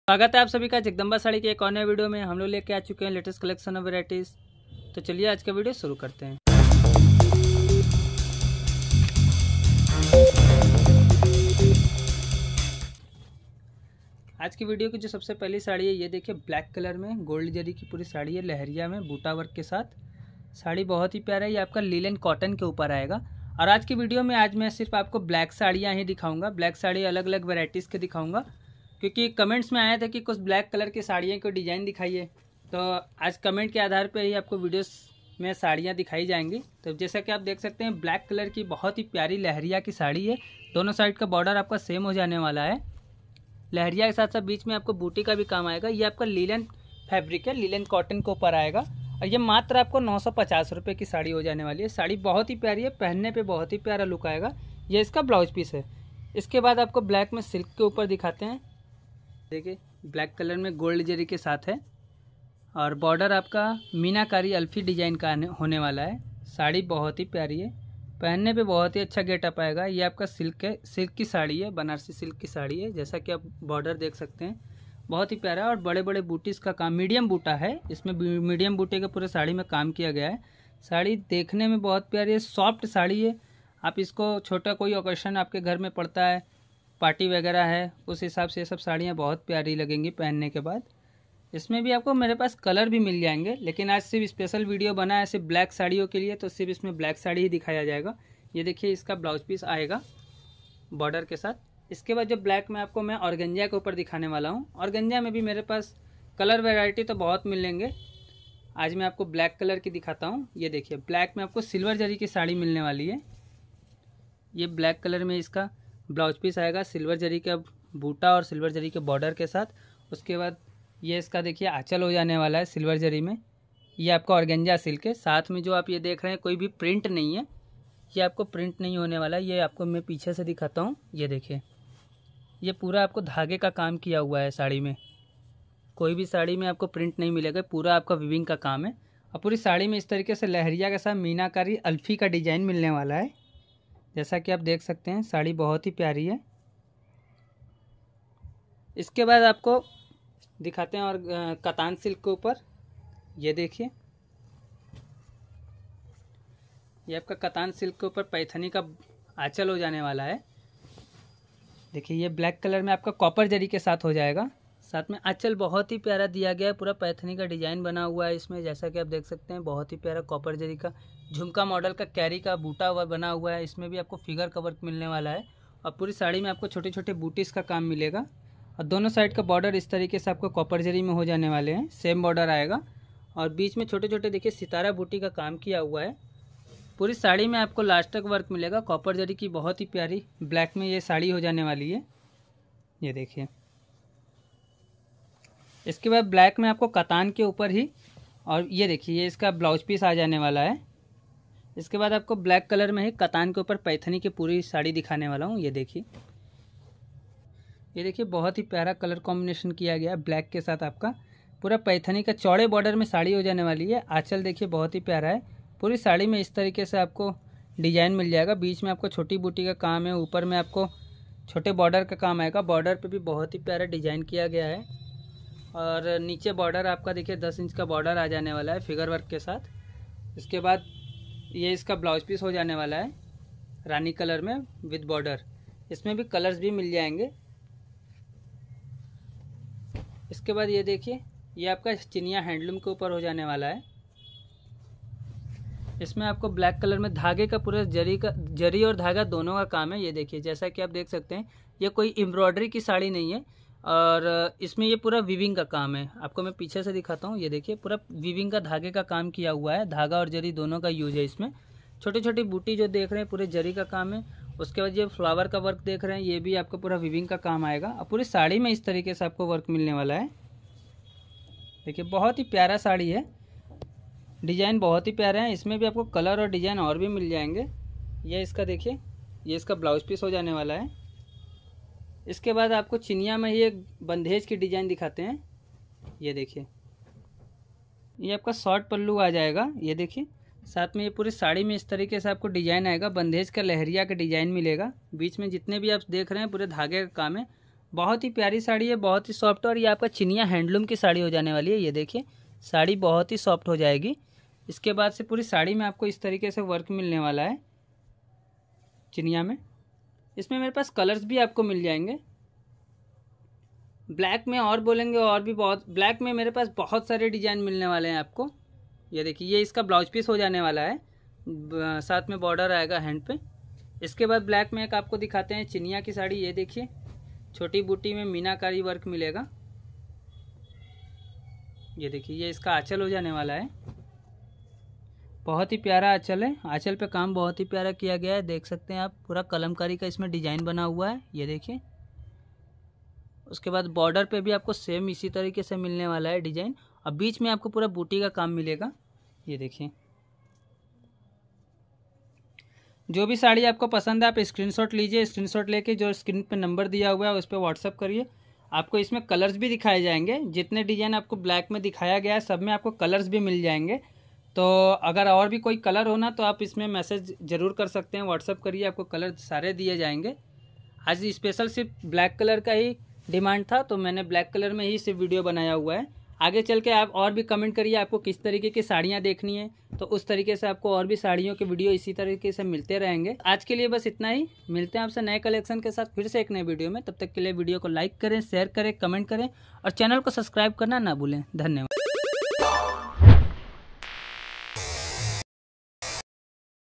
Banarasi Saree Buy Singal Piece At Wholesale Price 950₹Only |BlackSarees|Jagdambasareesvns#saree
Jagdambasareesvns Latest Saree Degine

In this I show Our lovely Coustomers Demanding Saree Only Black Colour Sarees